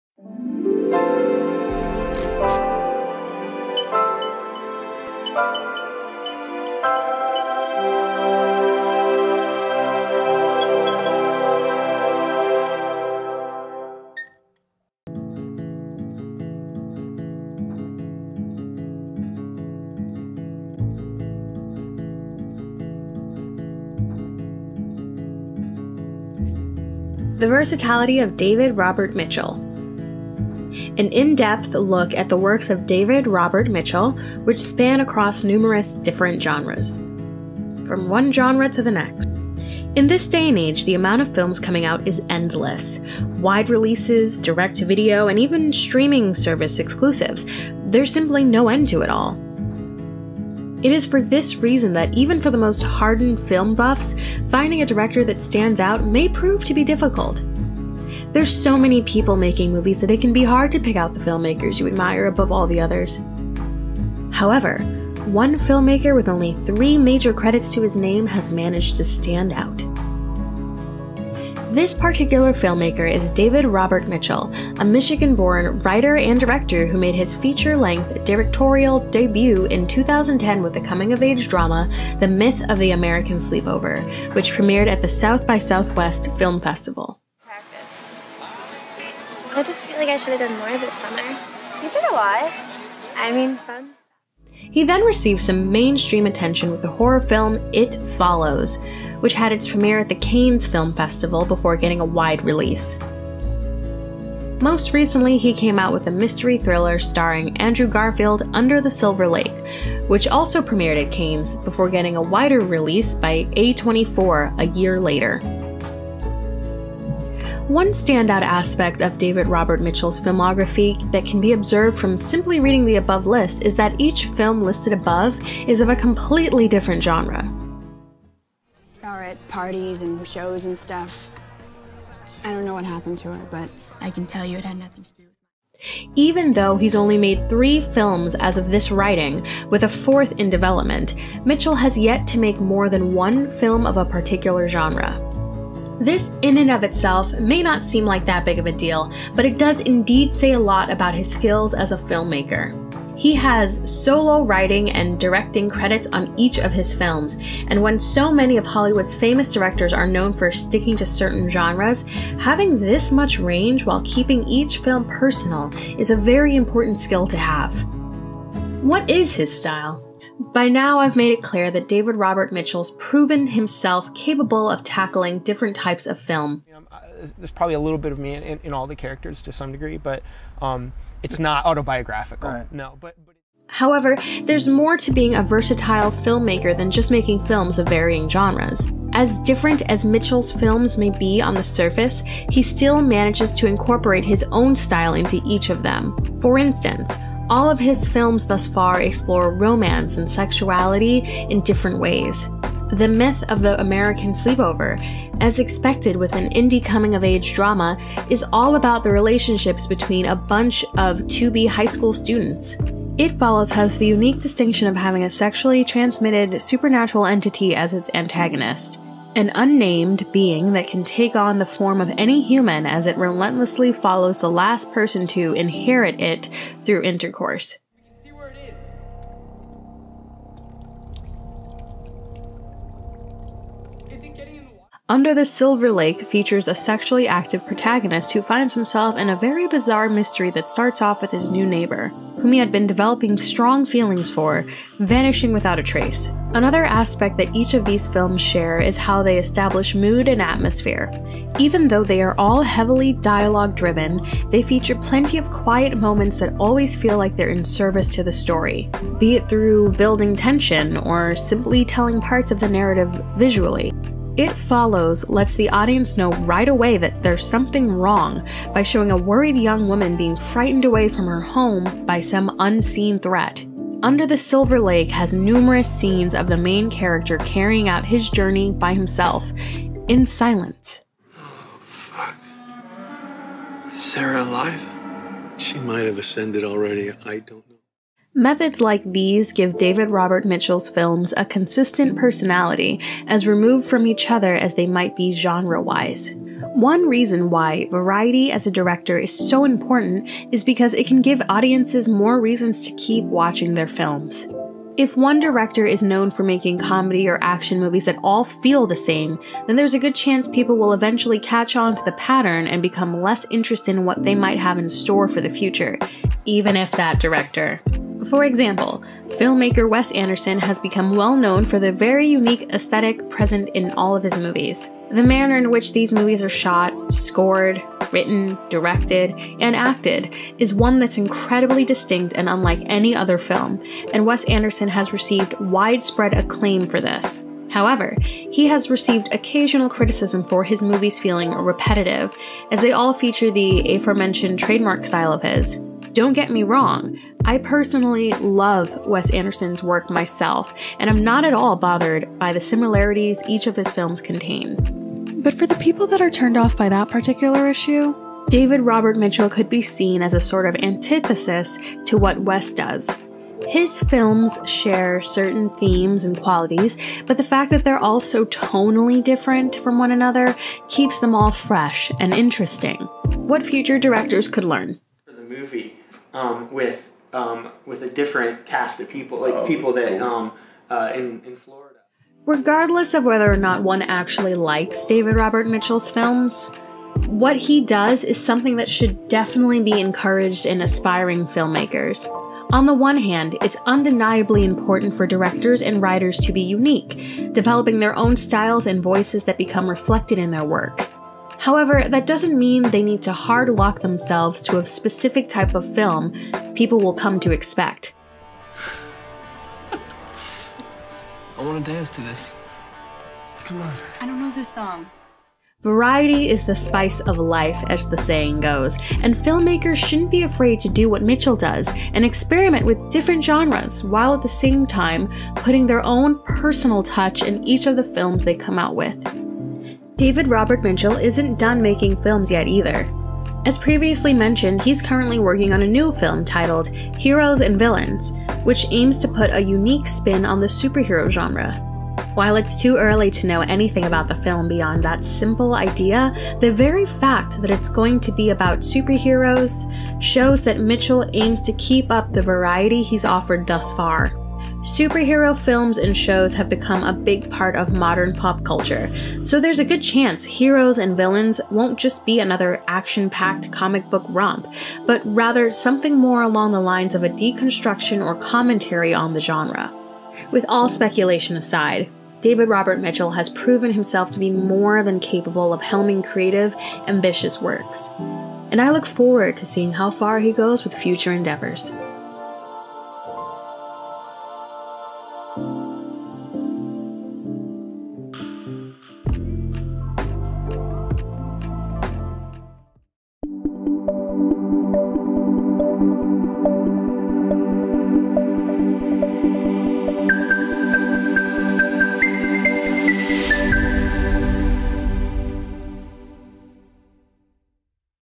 The Versatility of David Robert Mitchell: An In-Depth Look
An in-depth look at the works of David Robert Mitchell, which span across numerous different genres.

'Elvis' Series